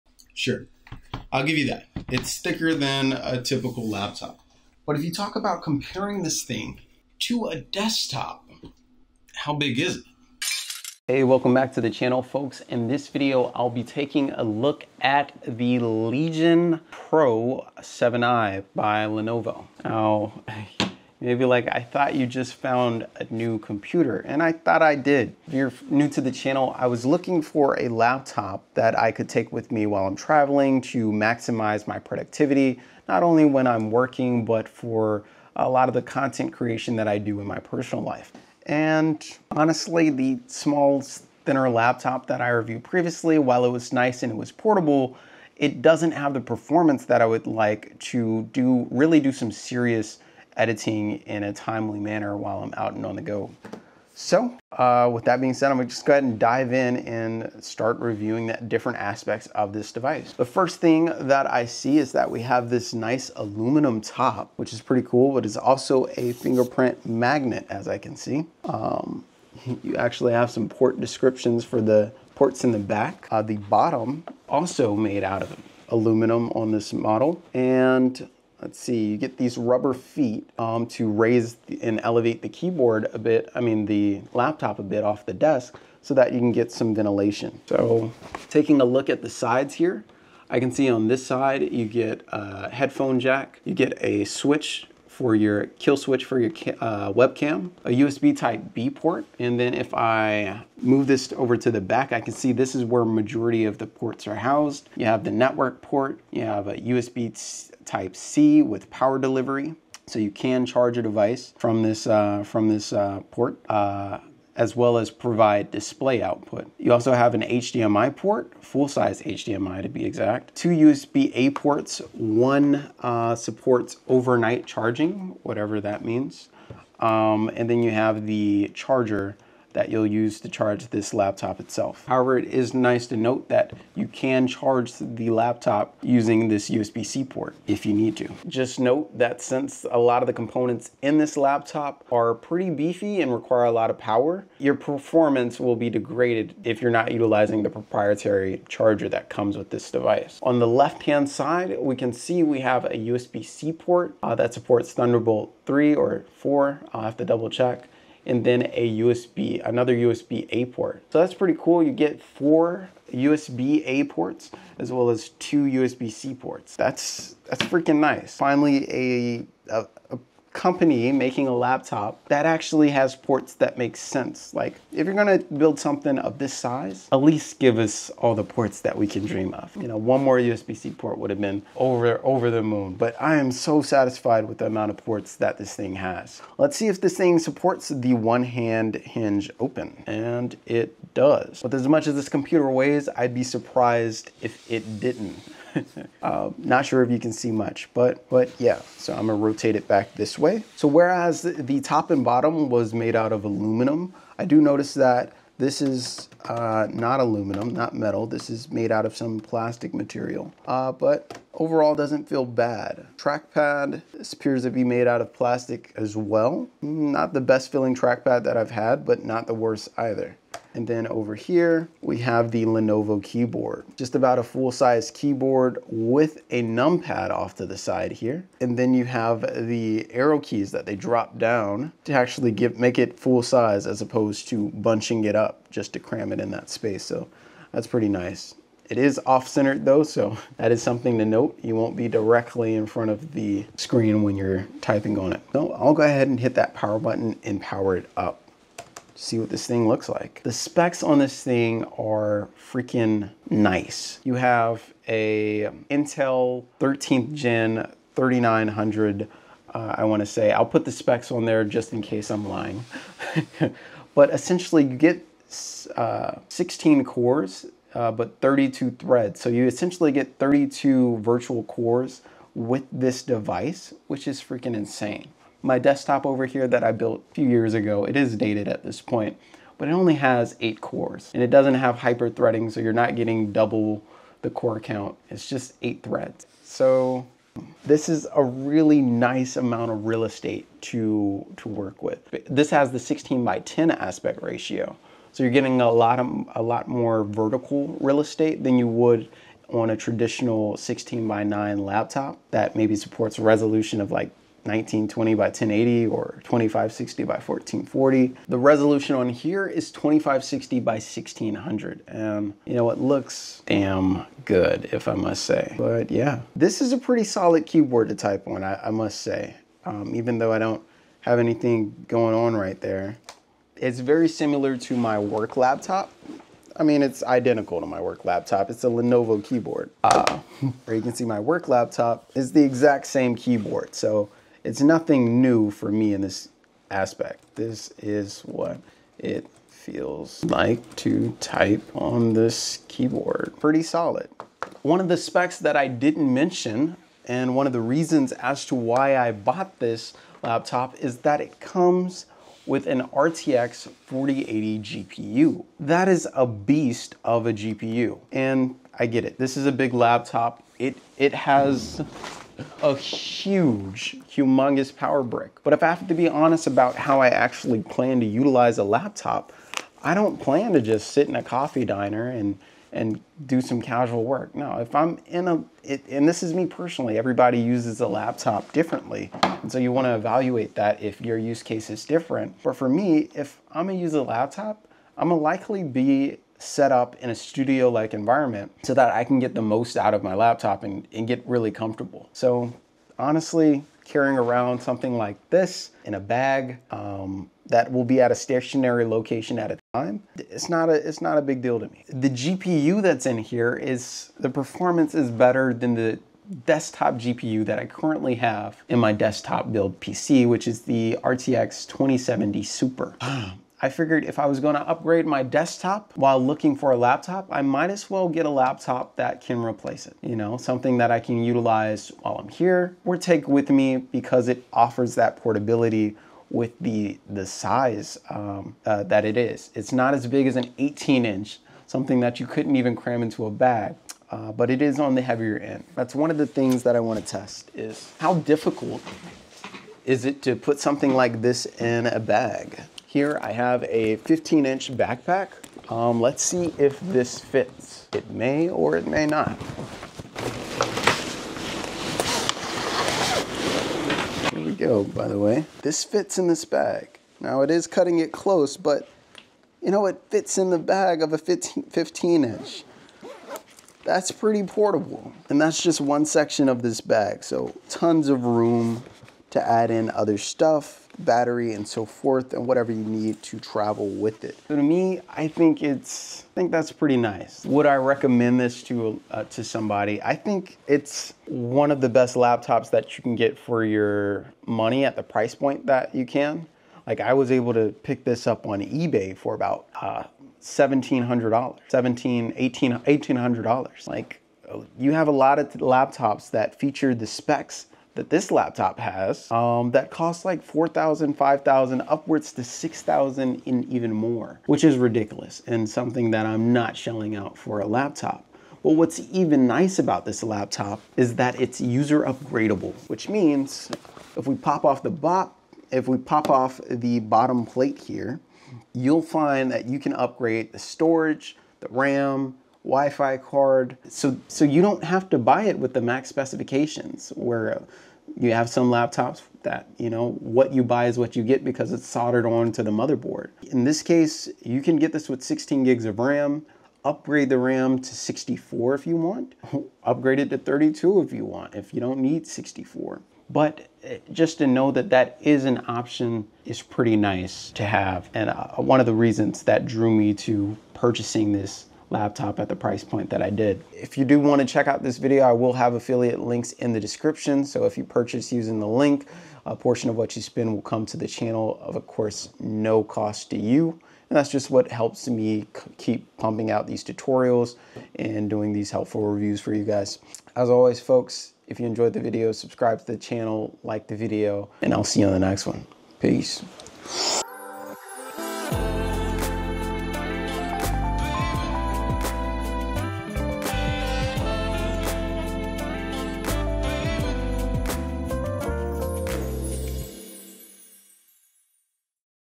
This might be the most UNDERRATED laptop for video editing: Legion Pro 7i
I've been searching for a laptop that could keep up with my content creation needs while traveling for work and came across the Lenovo Legion Pro 7i. In this video, I'll be going over the specs, the plethora of ports, and why it's a serious contender against many desktop setups. I'll also show you how this 16inch powerhouse fits perfectly into a travel bag without sacrificing performance. Stick around to find out why the Lenovo Legion Pro 7i could be the best investment for your editing workflow!

Lenovo Legion Pro 7i

Check this out on Amazon
eBay also usually has a good bit of deals

Timestamps:

0:00 - Intro
0:59 -  First impressions...
1:27 - Laptop IO
4:10 - Specs
7:15 - Bag Test
7:54 - Final Thoughts